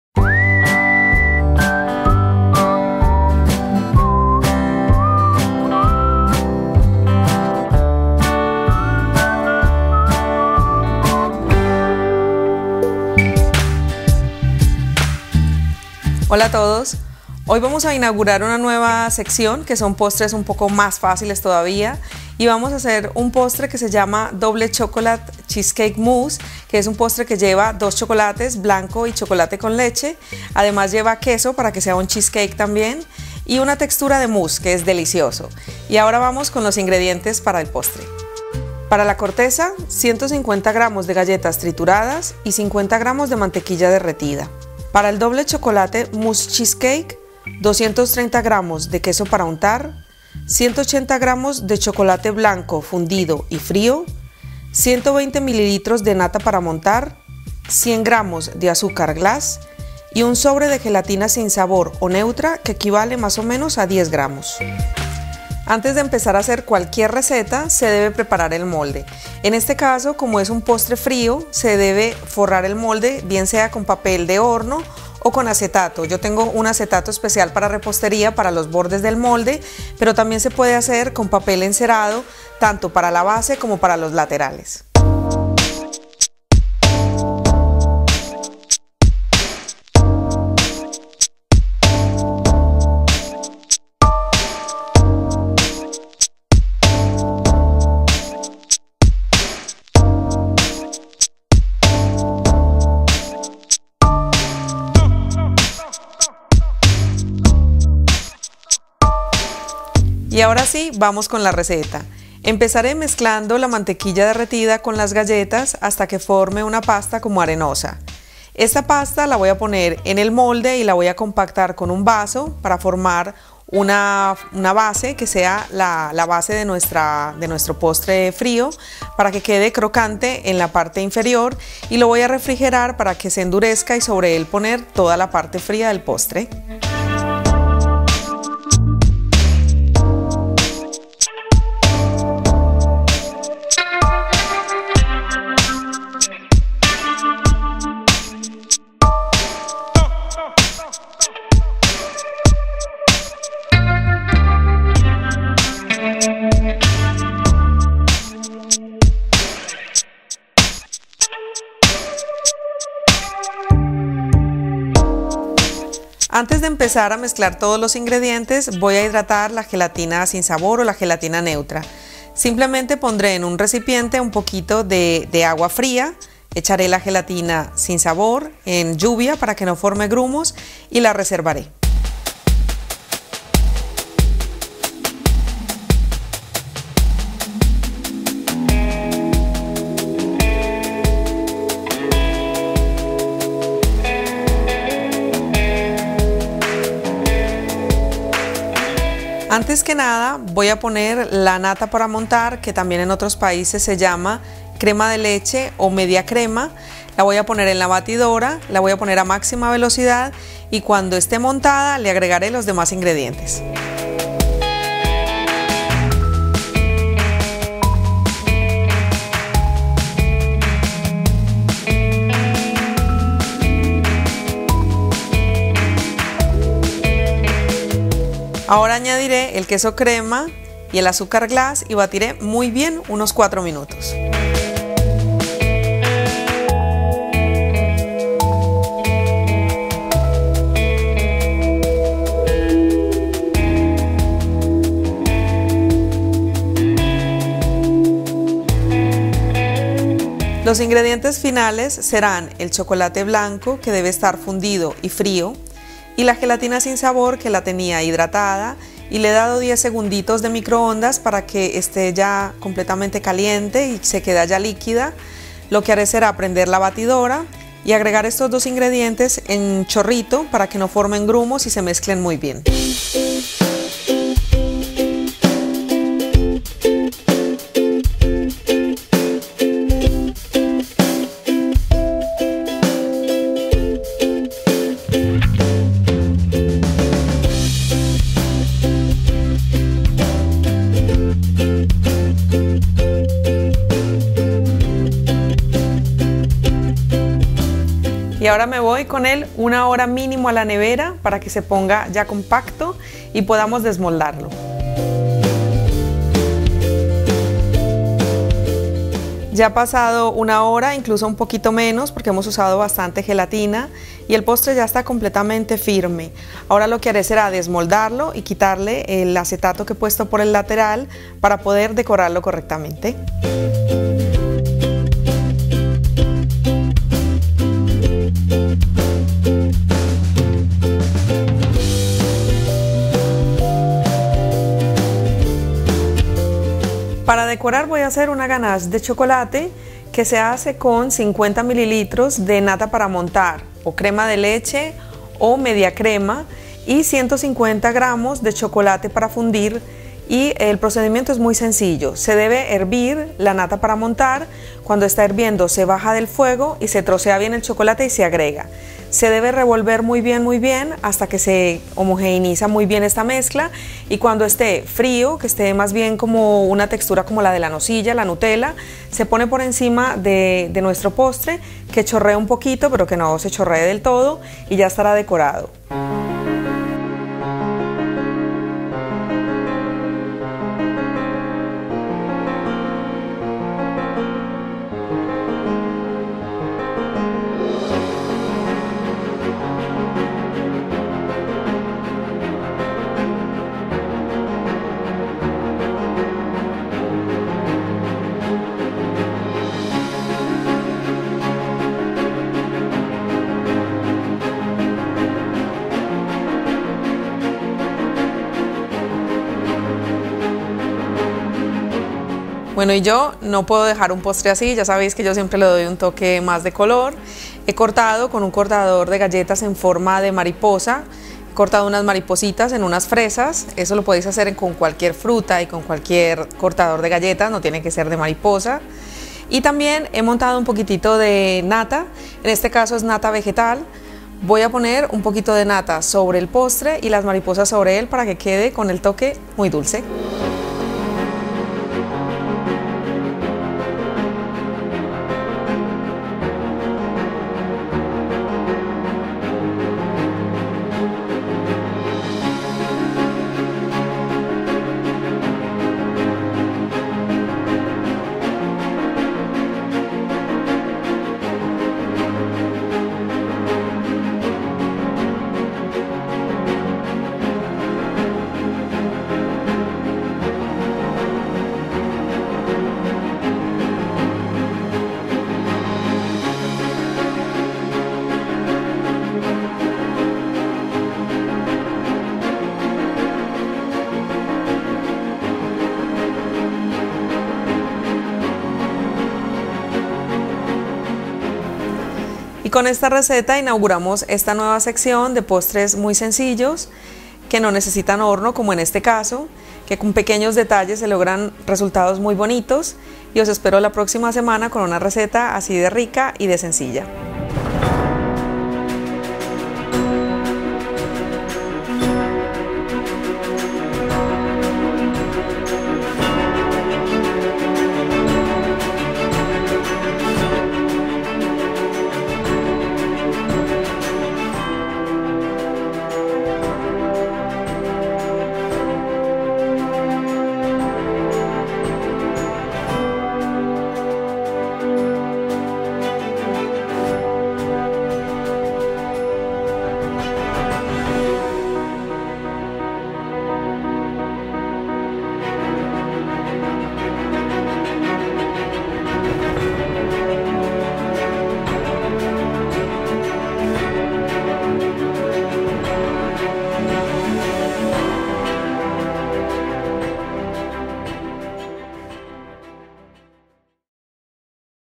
Doble Chocolate Cheesecake Mousse | Receta Fácil y Original
Inauguramos la sección de postres fáciles de hacer con esta receta original de doble chocolate cheescake mousse sin horno, una perfecta elección para darle un toque muy especial a tu mesa. Combina el suave chocolate blanco y el delicioso chocolate con leche, además en cada bocado se mezcla la frescura de una mousse con el sabor inconfundible del cheesecake. Vistoso y decorado con fresas para iniciarse en el mundo de la repostería.  Pruébalo, te sorprenderá.

INGREDIENTES

Para la corteza:

-150 gr de galletas trituradas
-50 gr. de mantequilla derretida

Para el doble chocolate cheesecake mousse:

-230 gr. de queso para untar
-180 gr. de chocolate blanco fundido y frío
-120 ml. de nata para montar (crema de leche o media crema)
-100 gr. de azúcar glas
-1 sobre de gelatina sin sabor neutra (10 gr.)

Para el ganache de chocolate:

-50 ml. de crema de leche
-150 gr. de chocolate para fundir

Para decorar:

-Nata montada (vegetal)
-Fresas frescas
-Cortador de galletas